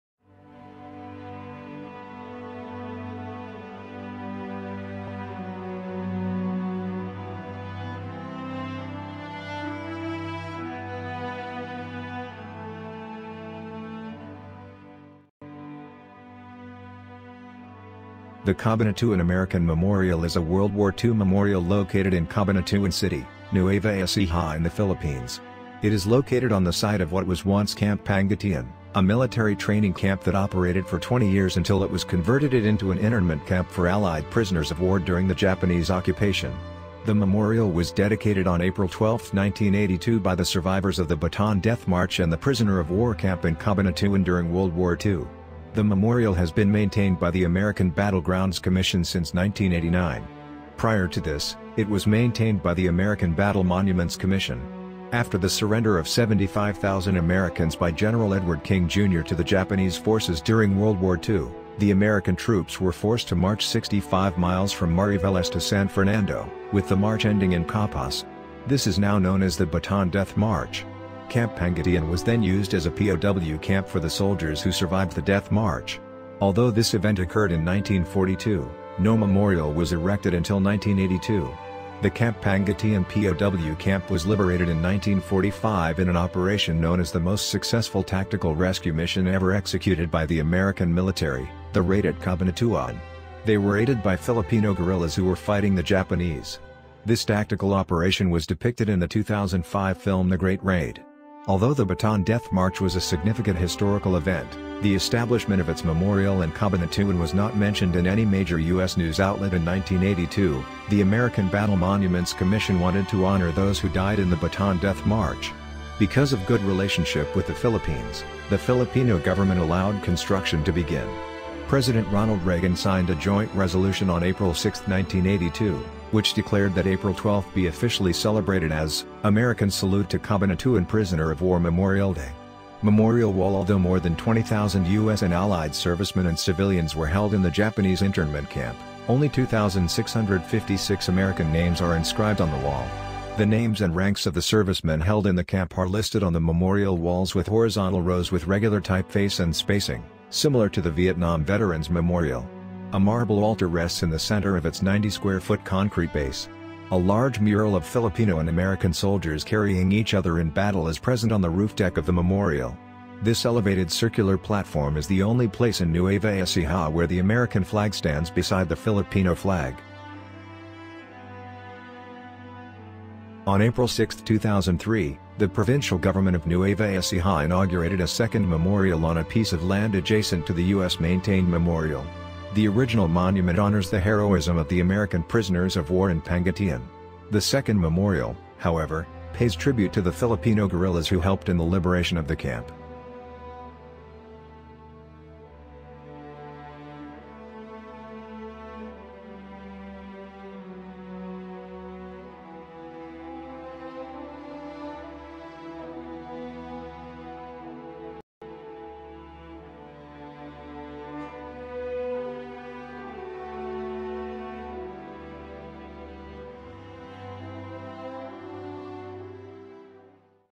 Cabanatuan American Memorial - Camp Pangatian
Camp Pangatian in Cabanatuan City, Nueva Ecija is the largest war memorial camp in the Philippines. The camp was originally a military training camp but it became a concentration camp for prisoners during the Japanese occupation of the Philippines.
Pangatian is a military camp where the Japanese Imperial Army imprisoned American and Filipino soldiers.